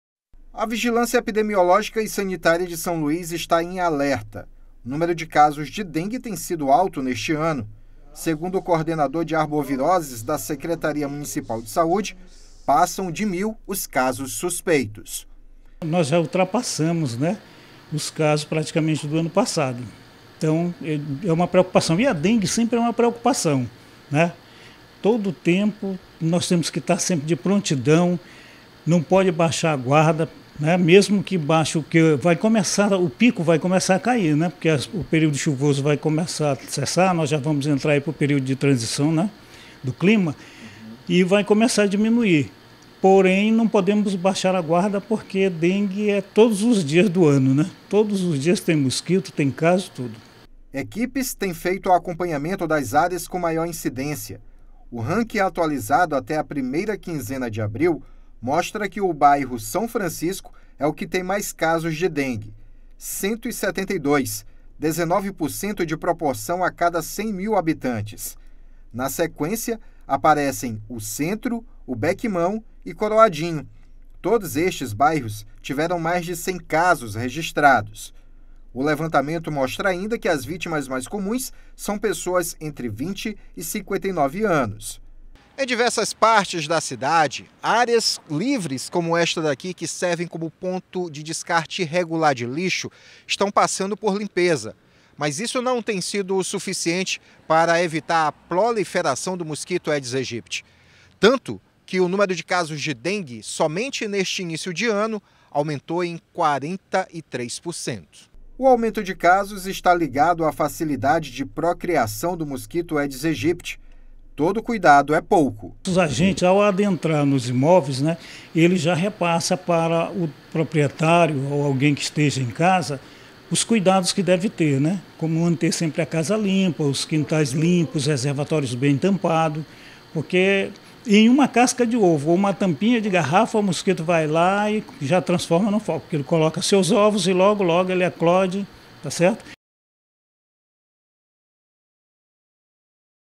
Agentes de saúde monitoram alta incidência de dengue em bairros de São Luís
A alta incidência de casos de dengue na capital maranhense tem preocupado autoridades do setor de saúde pública. Bairros de São Luís estão sob monitoramento de agentes. Os registros de caso suspeitos, chamam a atenção para o problema.